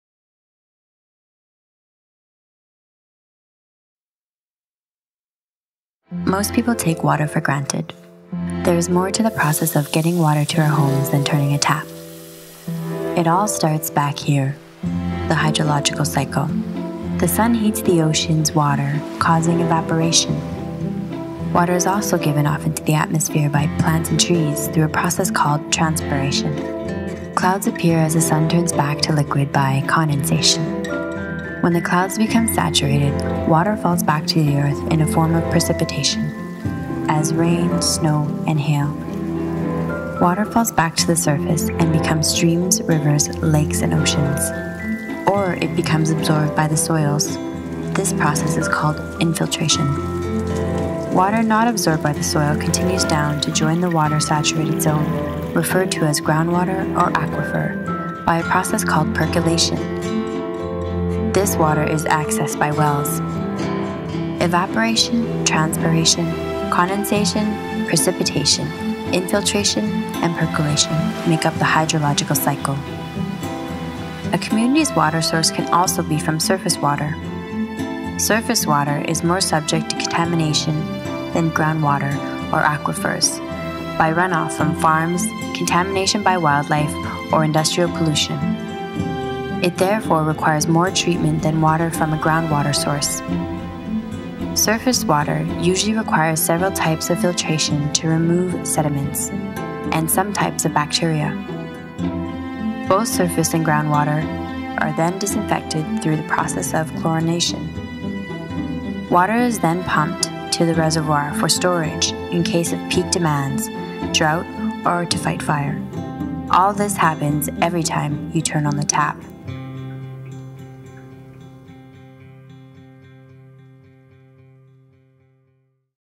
hydrological cycle Corporate video
Short video I made when I worked on Waterkeepers. I think the corporate training website has been deactivated. since I can't find it any where.